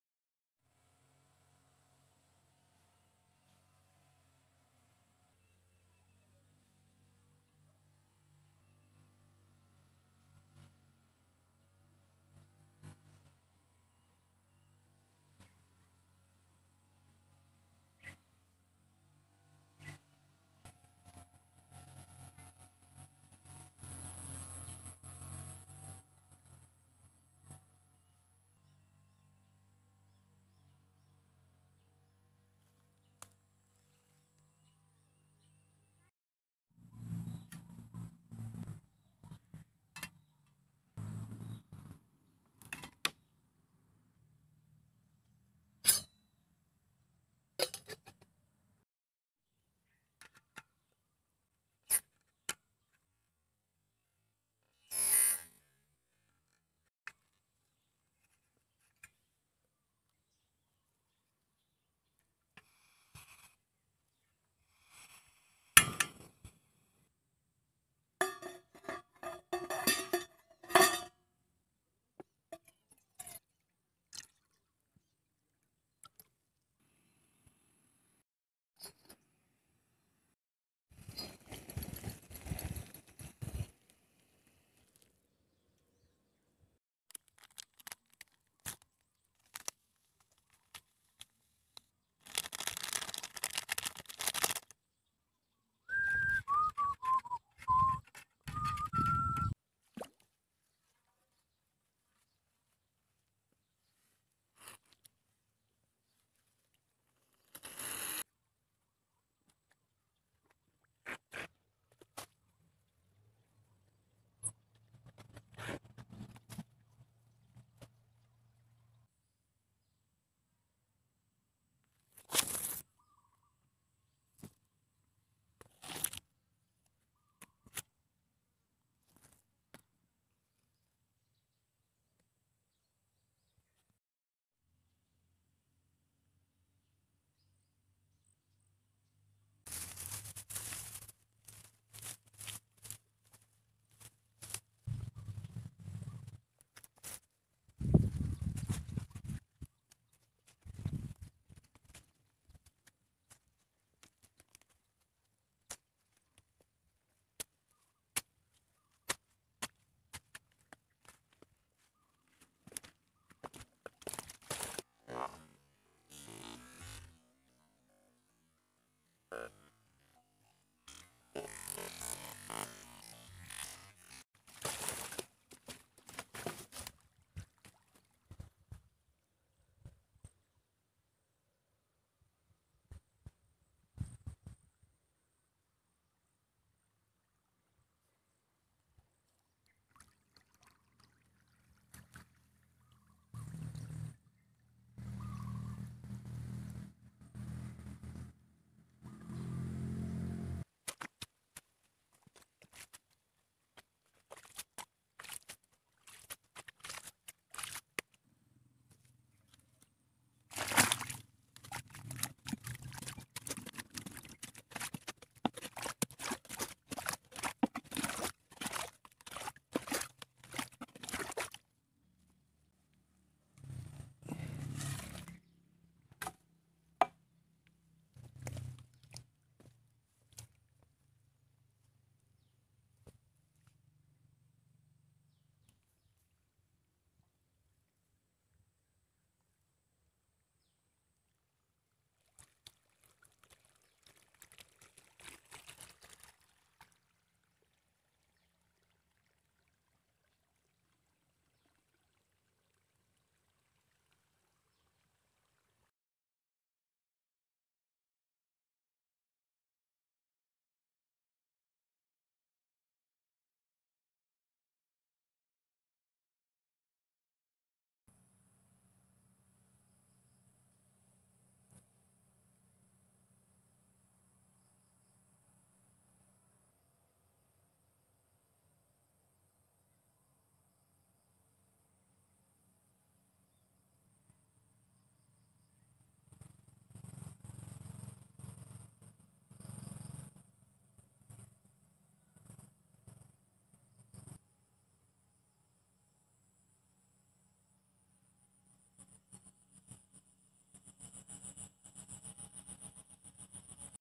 Gara2 Line "E" & tiada kerja. Video 3bulan lalu.
hai. syukur dapat kembali. video lama yg mau upload baru terlepas dr kandang hari ni. dalam video sya buat parit untuk mengalirkan air ke kolam besar yg sudah lama tiada air & tidak dapat menampung air. terima kasih atas sokongan anda. Undilah BAHAR.😂